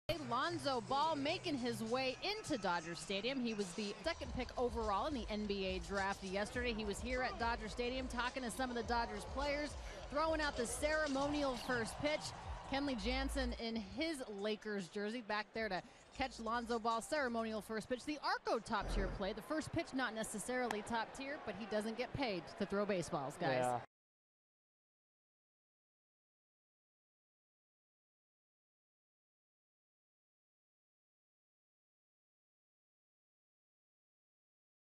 Lonzo Ball tosses first pitch
Lakers first round pick Lonzo Ball throws out the first pitch prior to the Dodgers' game against the Rockies